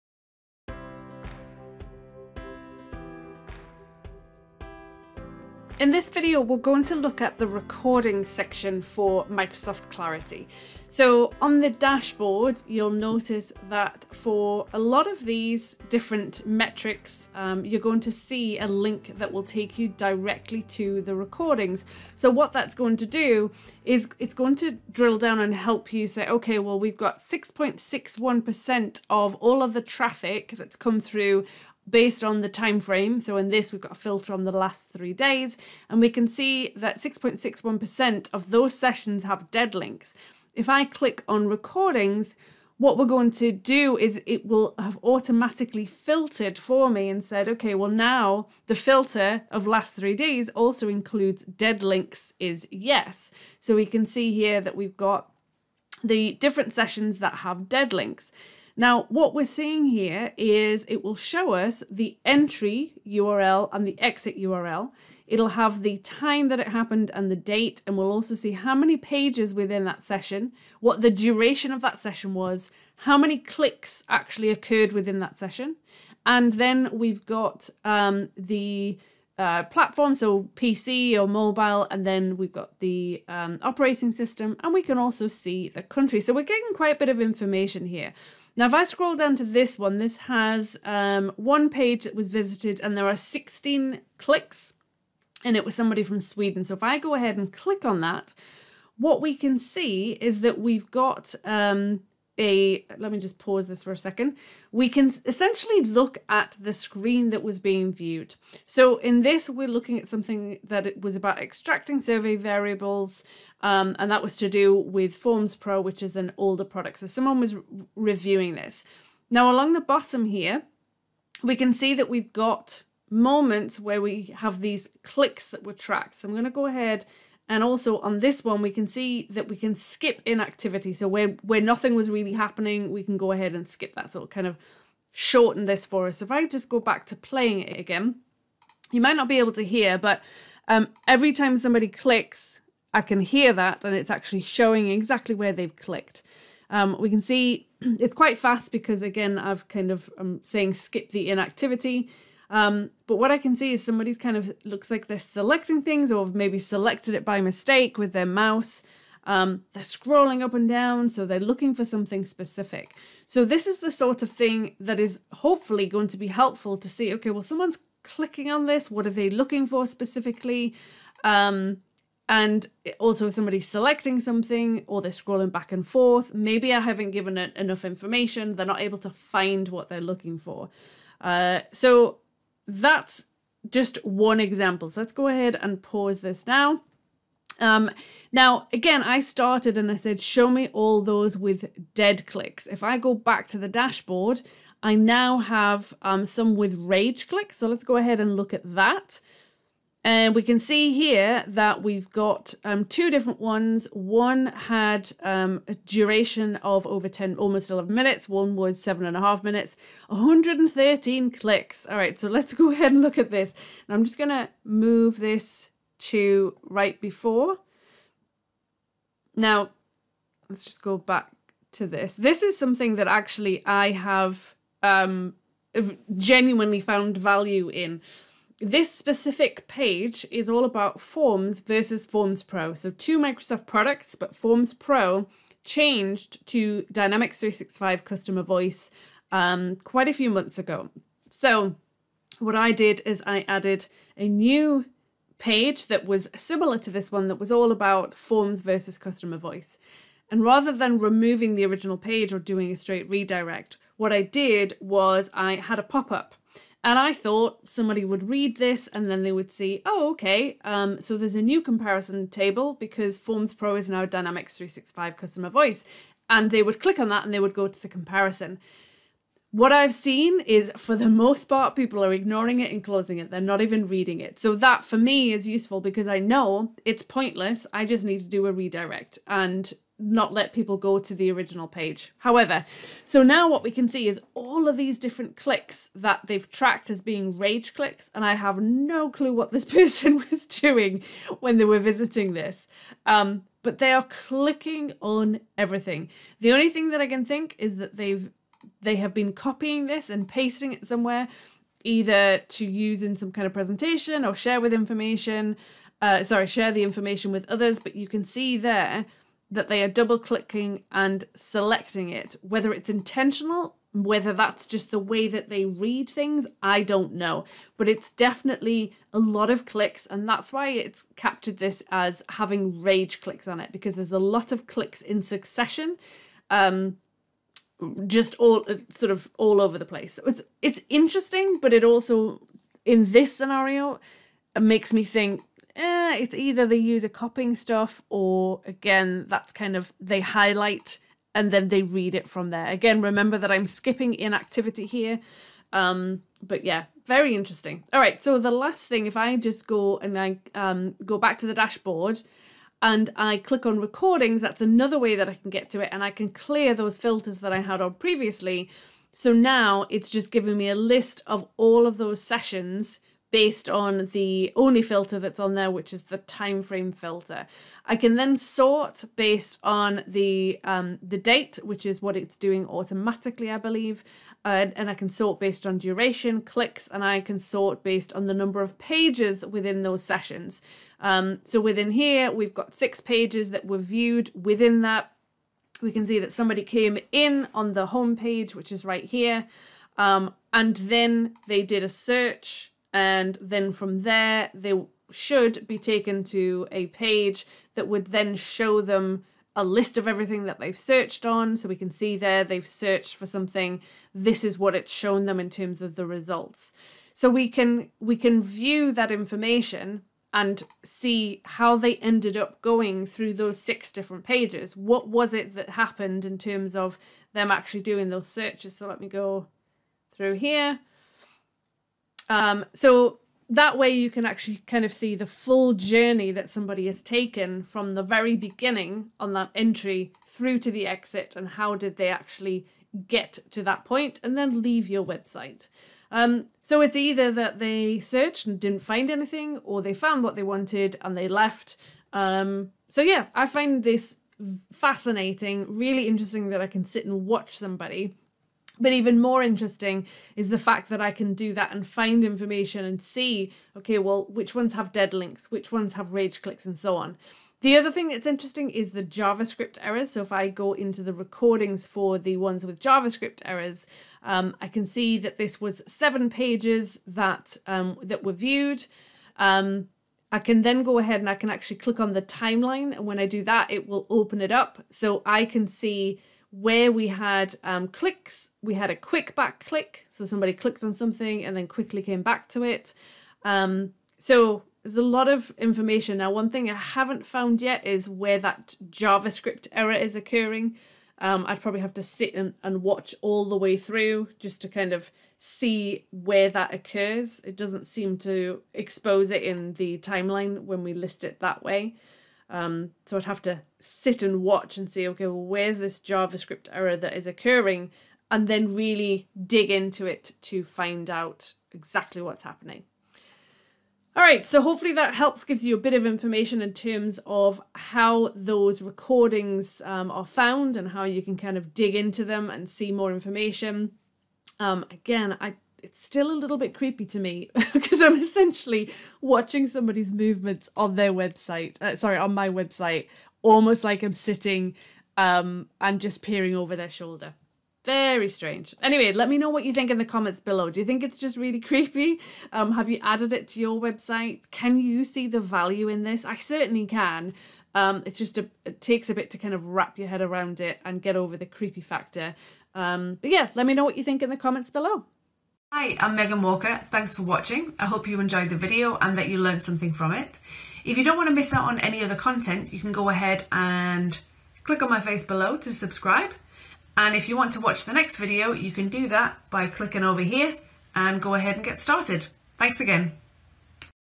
Reviewing Recordings In Microsoft Clarity - Microsoft Clarity Series
You've added the tracking code to your website, and you've been looking over the dashboard. now let's check out the recordings! A recording covers a session that occurred on your site. What's a session? It's defined as the amount of time a user spent on your site. A single user can open multiple sessions that can happen on the same day, or over several days, weeks, or months. A session ends if the user is inactive for 30 minutes. A recording covers a session and can include dead clicks, rage clicks, navigation to different pages etc. Let's check this out!